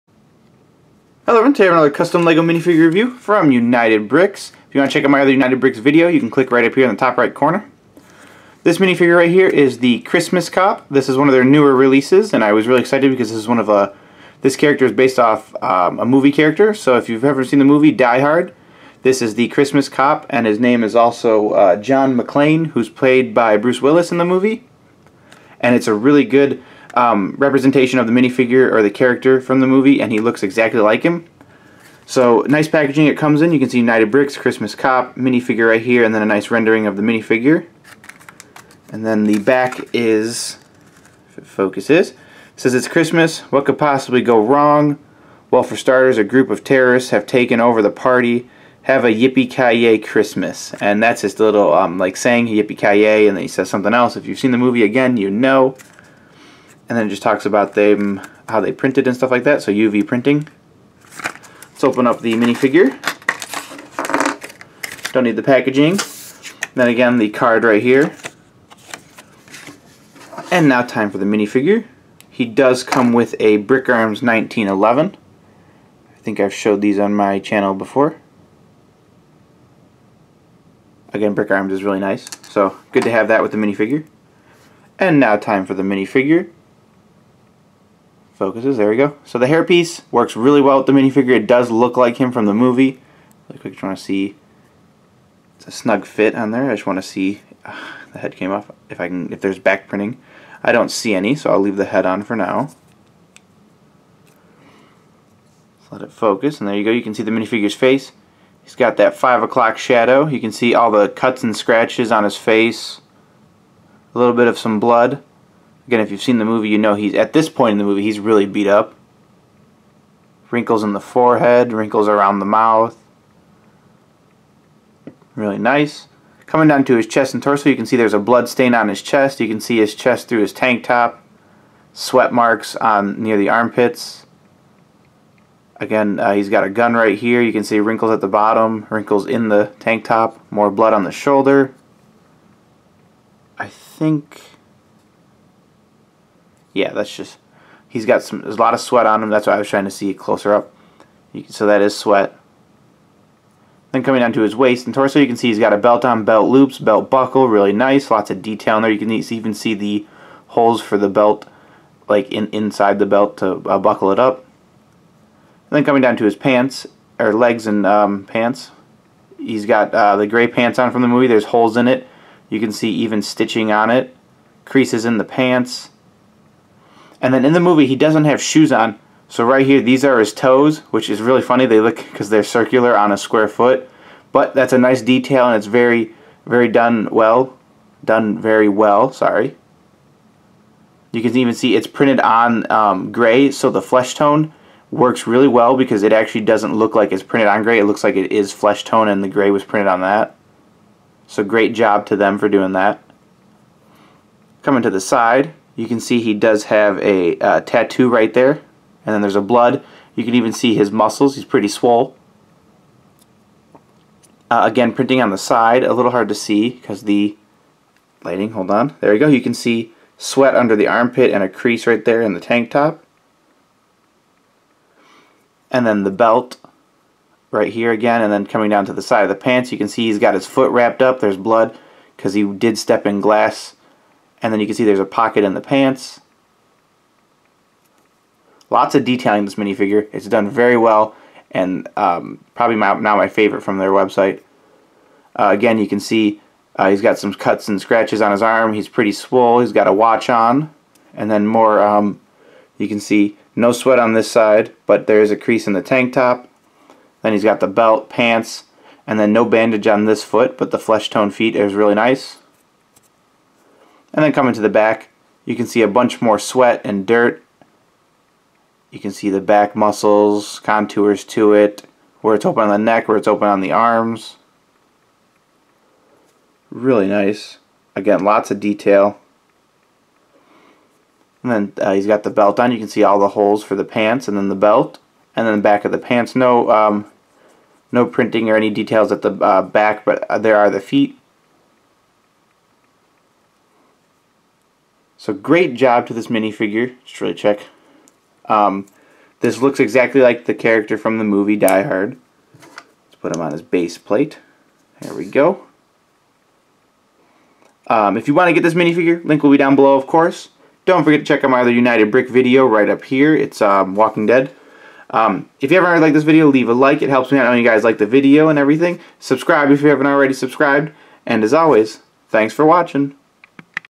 Custom Lego Minifigure Review: United-Bricks Christmas Cop
"Yippee Ki Yay Mother Bricker!"  The Christmas Cop is here to save Christmas!  Love getting movie characters turned into Legos.  So much detail is put into this minifigure from the sweat stains, to the feet, and even the blood.  As of now he is my favorite from United-Bricks!  If you love the Die Hard movies and love Legos then this minifigure is for you!